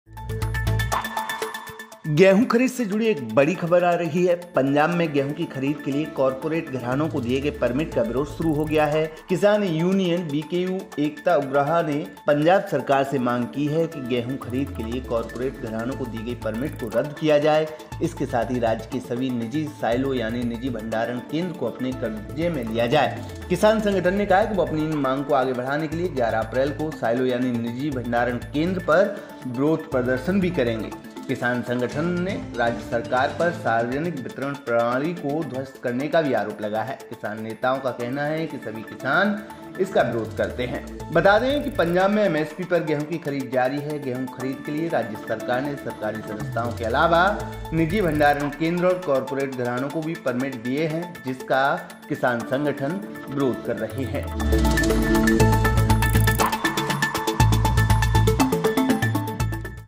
breakingnews | Wheat Procurement: Punjab में निजी साइलो और कॉरपोरेट घरानों को परमिट का विरोध शुरू
गेहूं की खरीद से जुड़ी एक बड़ी खबर आ रही है. पंजाब में गेहूं की खरीद के लिए कॉरपोरेट घरानों को दिए गए परमिट का विरोध शुरू हो गया है. किसान यूनियन बीकेयू (एकता उग्राहा) ने पंजाब सरकार से मांग की है कि गेहूं खरीद के लिए कॉरपोरेट घरानों को दी गई परमिट को रद्द करने की मांग की है. इसके साथ ही राज्य के सभी निजी साइलो यानी निजी भंडारण केंद्र को अपने कब्जे में लेने का अनुरोध किया है. किसान संगठन ने कहा कि वो अपनी इन मांगों को आगे बढ़ाने के लिए 11 अप्रैल को साइलो  यानी निजी भंडारण केंद्र  पर विरोध प्रदर्शन भी करेंगे.